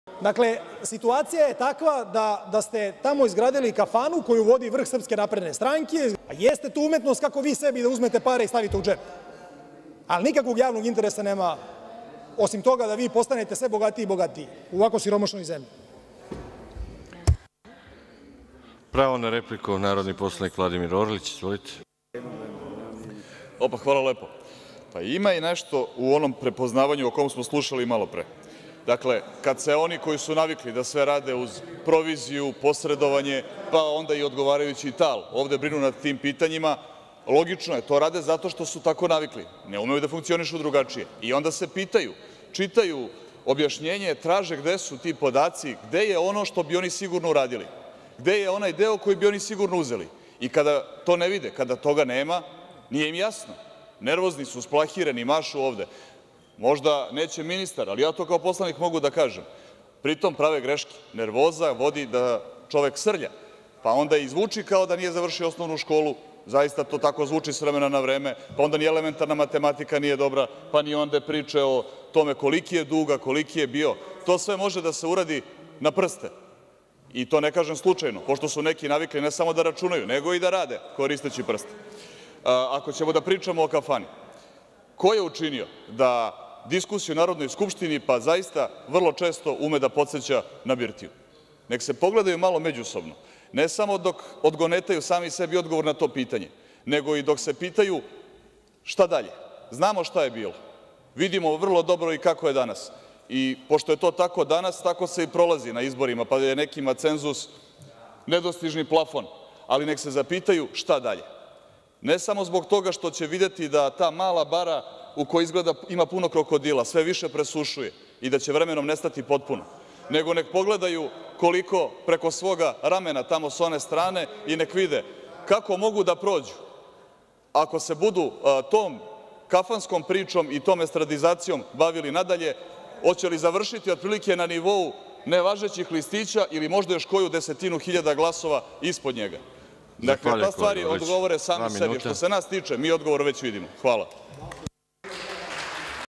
SNS Cukarica - Dr Vladimir Orlic vs Balsa Bozovic: Replika, 23.05.2017.
Narodni poslanik Srpske napredne stranke sa Cukarice, dr Vladimir Orlic: izlaganje na sednici Narodne skupstine Republike Srbije, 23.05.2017. godine.